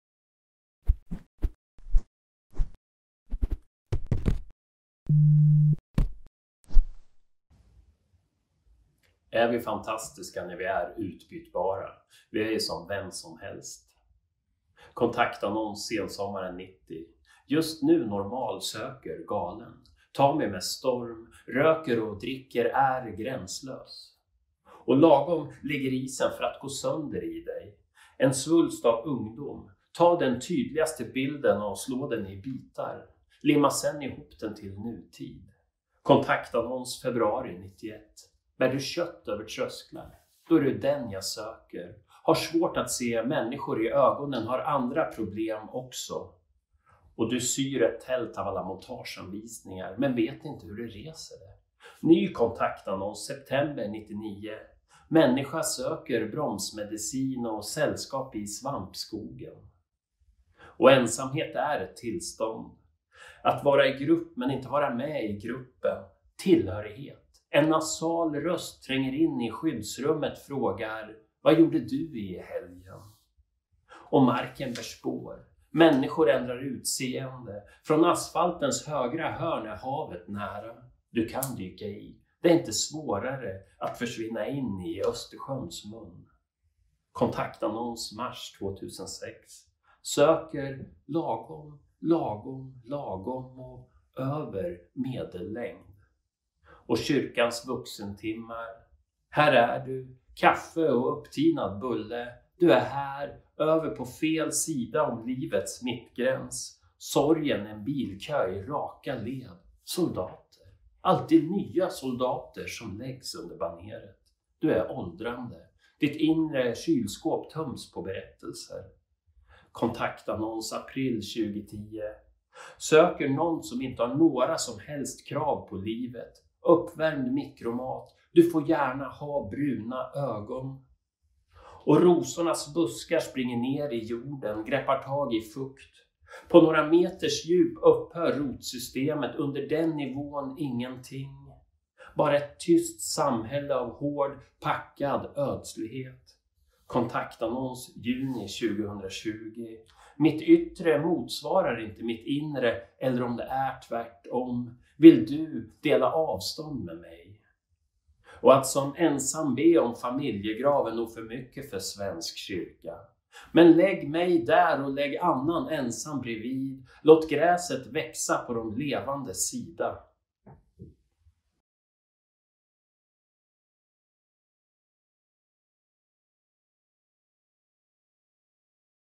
LITTERATUR SORMLAND ERIKSSON 4904 mp4
Christer Eriksson är en av tio poeter som medverkade i Folk och Kultur 2021 under vinjetten Sörmlandspoesi med...